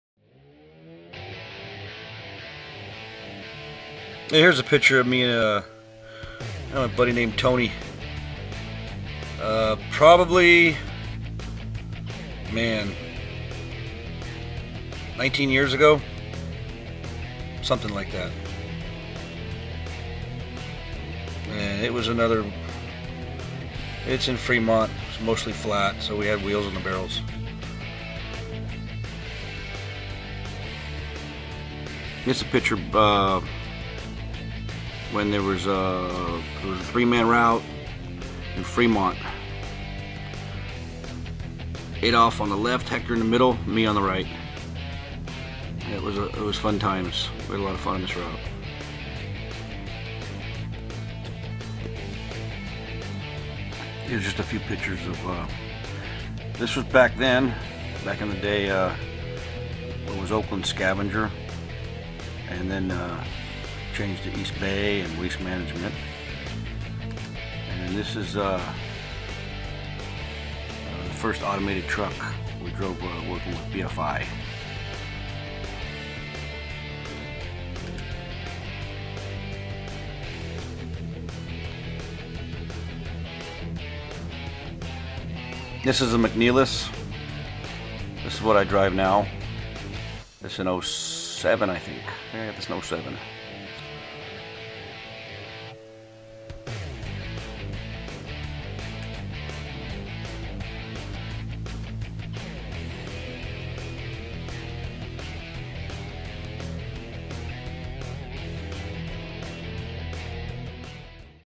garbage pictures then and now
some pictures of  me and my fellow trashmen